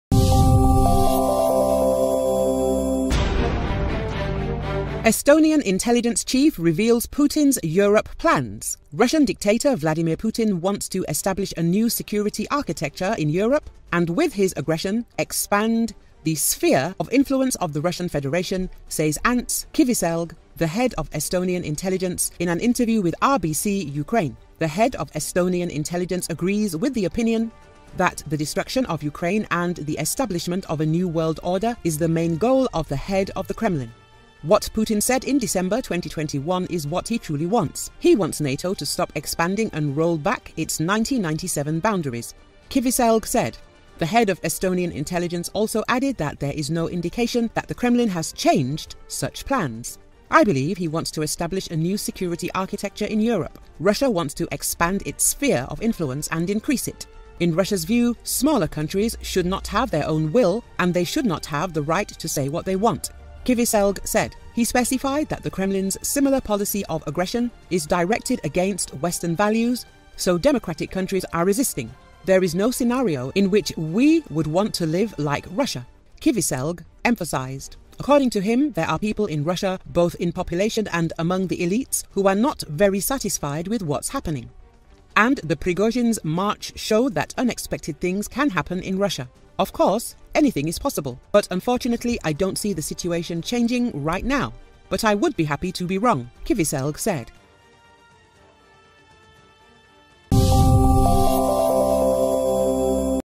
Estonian intelligence chief reveals Putin's Europe plans
Russian dictator Vladimir Putin wants to establish a new security architecture in Europe, and with his aggression expand the sphere of influence of the Russian Federation, says Ants Kiviselg, the head of Estonian intelligence, in an interview with RBC-Ukraine.
The head of Estonian intelligence agrees with the opinion that the destruction of Ukraine and the establishment of a new world order is the main goal of the head of the Kremlin.
"What Putin said in December 2021 is what he truly wants. He wants NATO to stop expanding and roll back to its 1997 boundaries," Kiviselg said.
The head of Estonian intelligence also added that there is no indication that the Kremlin has changed such plans.
"I believe he wants to establish a new security architecture in Europe. Russia wants to expand its sphere of influence and increase it. In Russia's view, smaller countries should not have their own will, and they should not have the right to say what they want," Kiviselg said.
He specified that the Kremlin's similar policy of aggression is directed against Western values, so democratic countries are resisting
"There is no scenario in which we would want to live like Russia," Kiviselg emphasized.
According to him, there are people in Russia, both in population and among the elites, who are not very satisfied with what's happening. “And the Prigozhin's March showed that unexpected things can happen in Russia. Of course, anything is possible, but unfortunately, I don't see the situation changing right now. But I would be happy to be wrong”, Kiviselg said.